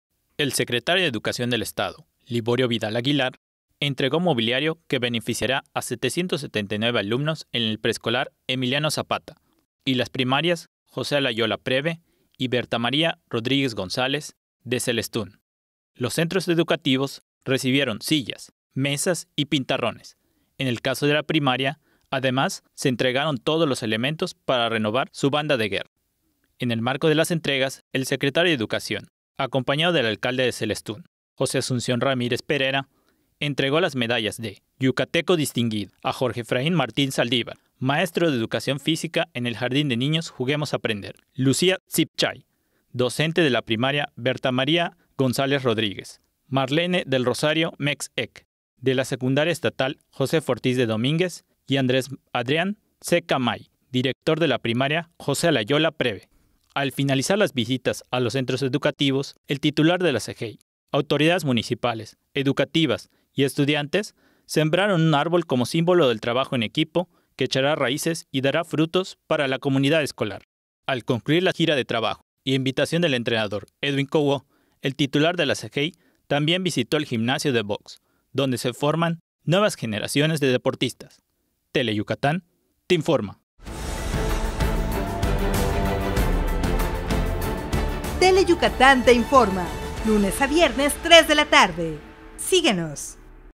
Niñas y niños de Celestún reciben mobiliario escolar | TELE Yucatán
Niñas y niños de Celestún reciben mobiliario escolar